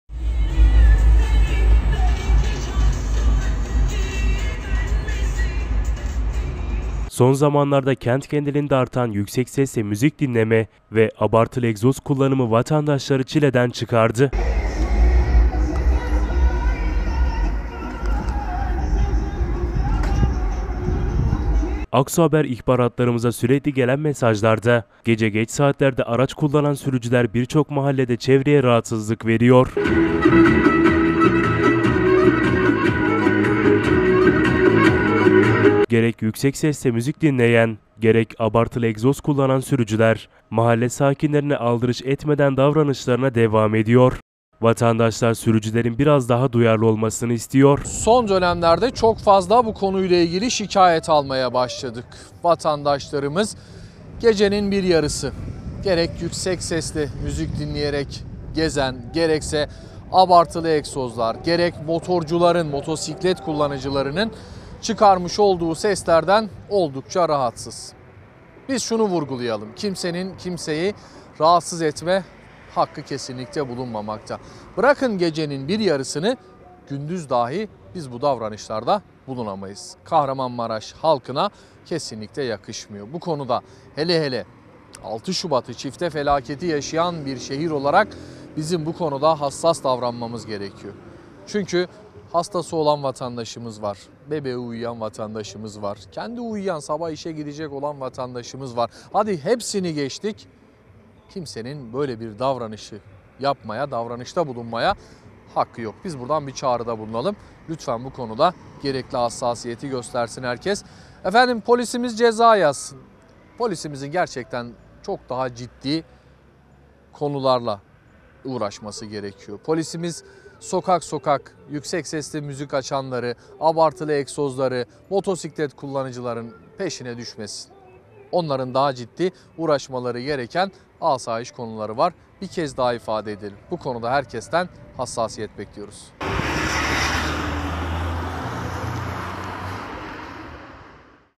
Yeter Artık!!!!
Son zamanlarda kent genelinde artan yüksek sesle müzik dinleme ve abartılı egzoz kullanımı vatandaşları çileden çıkardı.
Aksu Haber İhbar hatlarımıza sürekli gelen mesajlarda, Gece geç saatlerde araç kullanan sürücüler birçok mahallede çevreye rahatsızlık veriyor.
Gerek yüksek sesle müzik dinleyen gerek abartılı egzoz kullanan sürücüler, mahalle sakinlerine aldırış etmeden davranışlarına devam ediyor. Vatandaşlar sürücülerin biraz daha duyarlı olmasını istiyor.